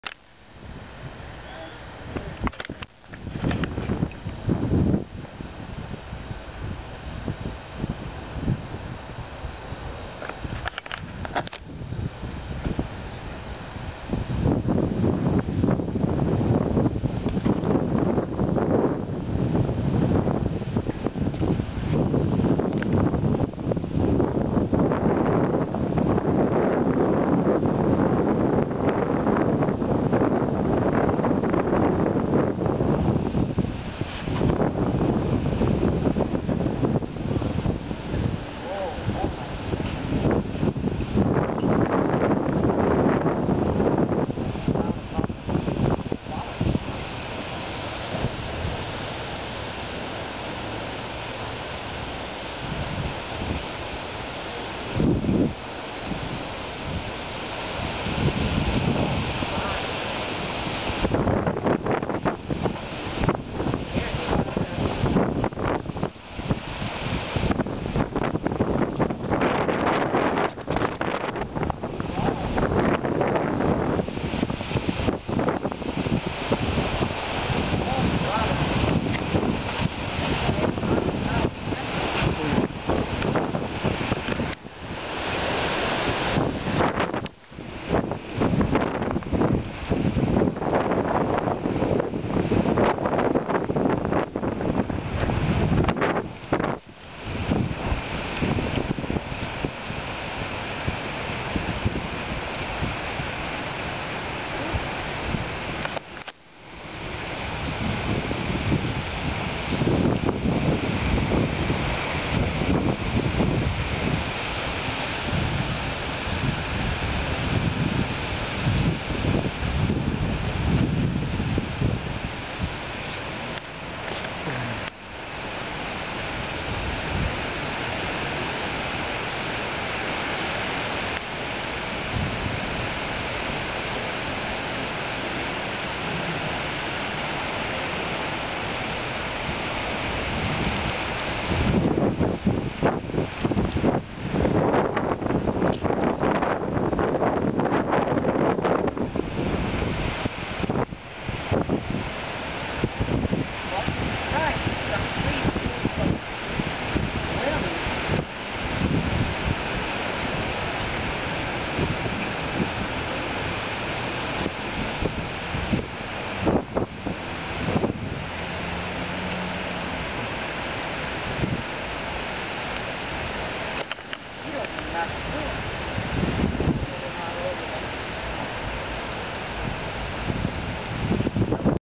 Lightning South of My HOme
We were under a Severe Thunderstorm Warning around 8:35 PM on Sept. 4th 2012. You can see how sharp some of the lightning is, and how wild some of the clouds look. You may hear the wind pick up and possibility hearing my mother talking. I did rotate a bit so you can see lightning from the southeast and southwest. Also the wind really started to pick up around 2:00 or so and it did kinda push me a little bit. IT also started to rain at the conclusion of this video. Although you didn't hear any thunder, there was a little bit of thunder later on that evening.